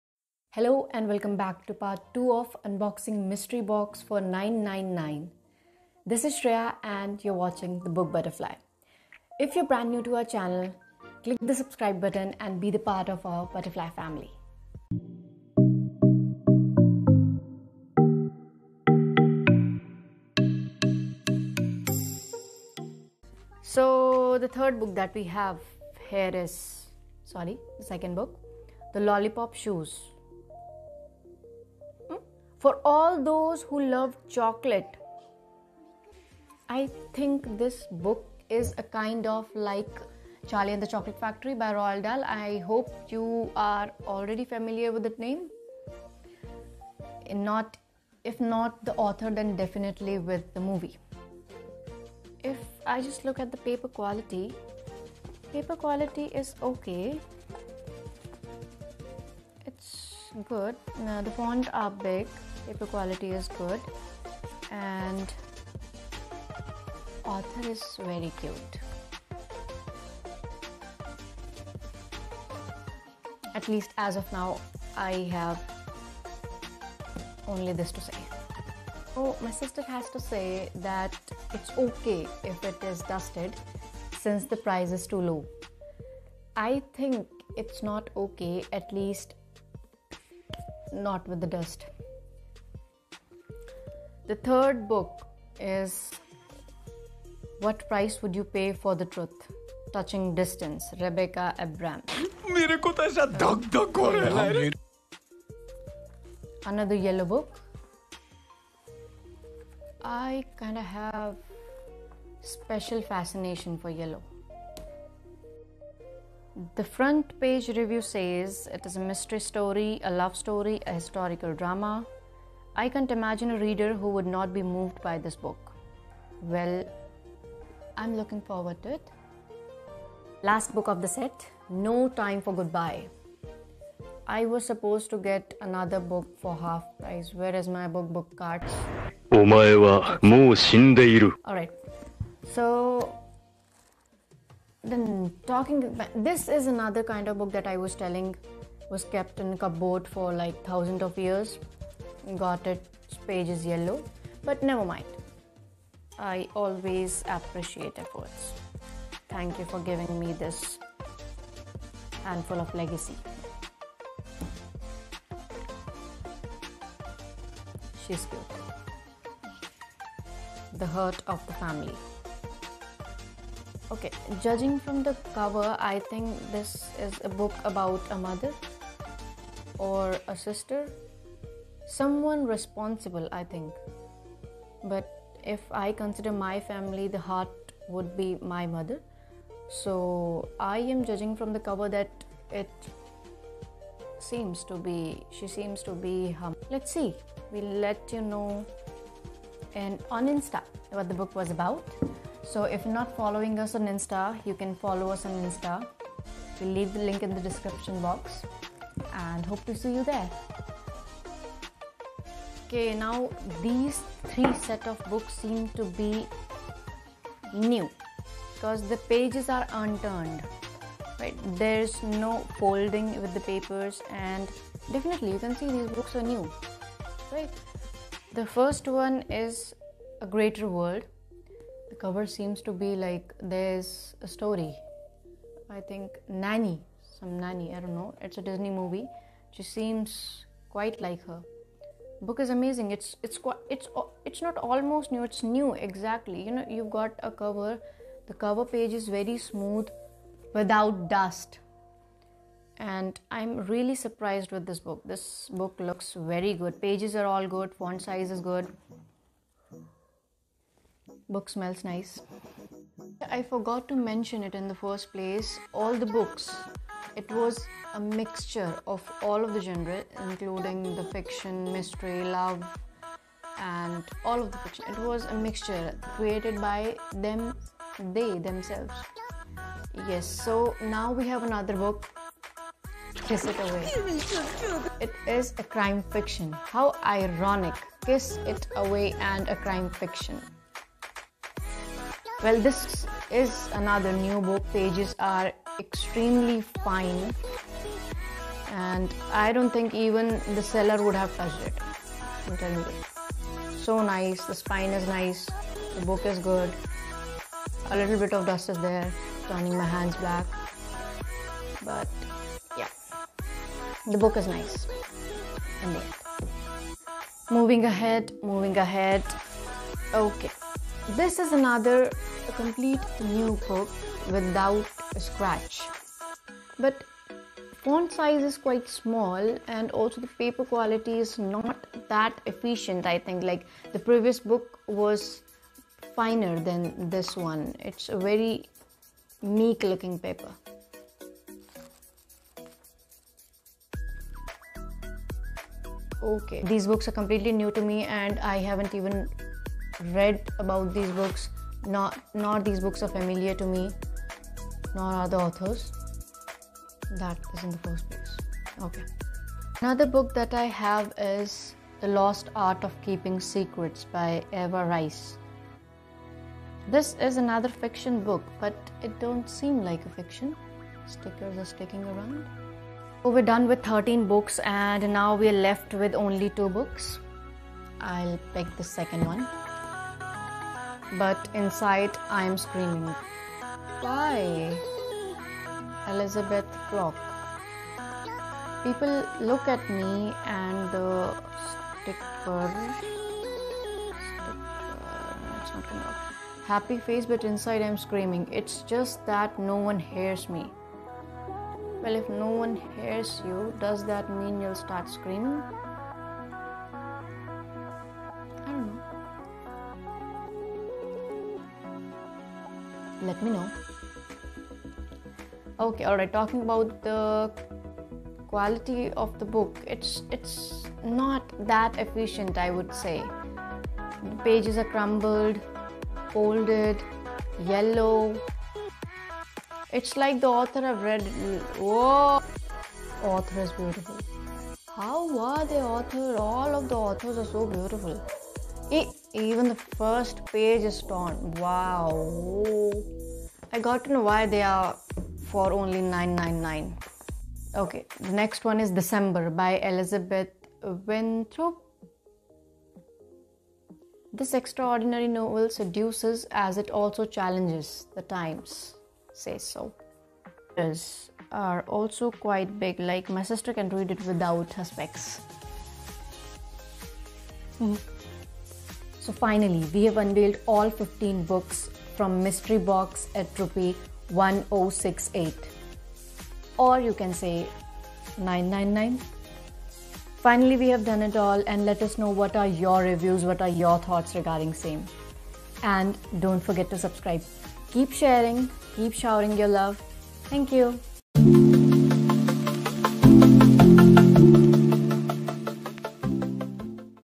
Unboxing Mystery Box || Part 2 ||99bookscart mystery box
This is a channel about book reviews ,recomendations and also suggestions. we are Shreya & Siddhi introducing you to our new channel 'The book butterfly'.
This is our second unboxing video of mystery box from 99bookscart.com that we purchased at only 1068
✨ You can use this code to get discount of 10%

. hope you enjoy the video and comment down your favourite part.